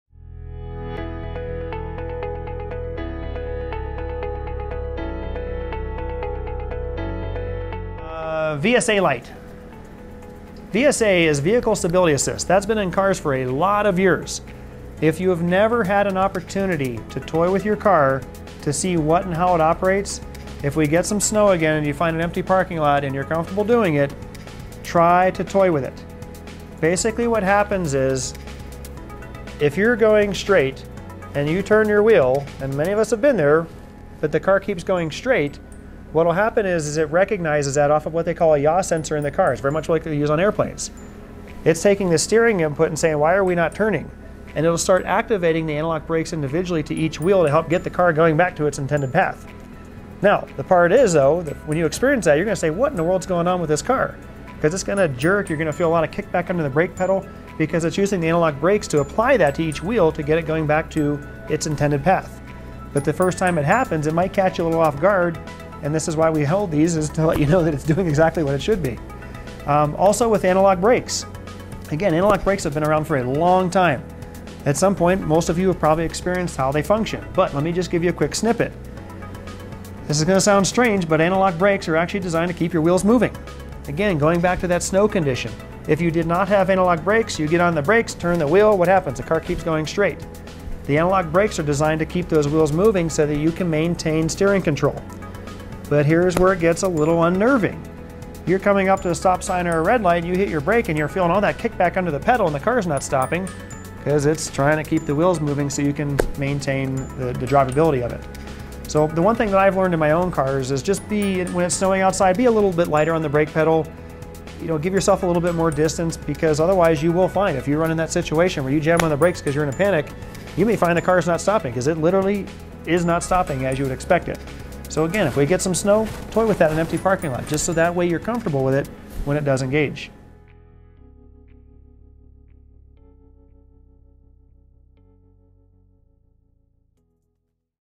VSA and ABS Systems Explained
The VSA (Vehicle Stability Assist) and ABS (Anti-lock Braking System) are two safety features found in most modern cars, but what do they actually do? It is important to know how the systems work so you can be aware in adverse conditions while driving.

In this video we will go over:
- How does VSA (Vehicle Stability Assist) work
- How does ABS (Anti-lock Braking System) work
- What to look for when these systems engage

Visit us today:
6025 Arapahoe Rd